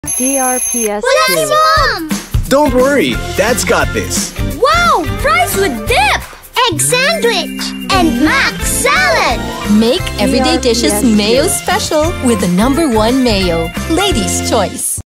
Lady's Choice Real Mayonnaise Radio Ad 2H 2023-2024 15s
Ito po ang 2H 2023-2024 Radio Ad ng Lady's Choice Real Mayonnaise - ang nagungunang mayo brand sa merkado!

DRPS Two 6190: Lady's Choice Real Mayonnaise Radio Ad 2H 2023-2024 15s